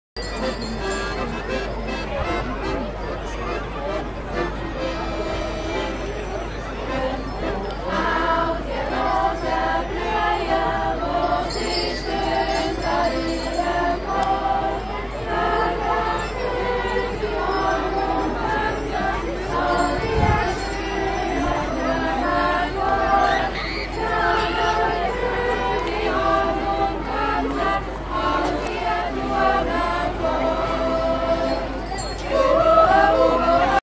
スイス発 子供ヨーデル：2017①チューリッヒ ゼクセロイテン1日目【スイス情報.com】
子供たちによるヨーデルの歌声です。

スイスの最大都市チューリッヒの最大イベント、 ゼクセロイテン (Sechseläuten) の一日目、子供パレードの様子を配信しています。

T.T.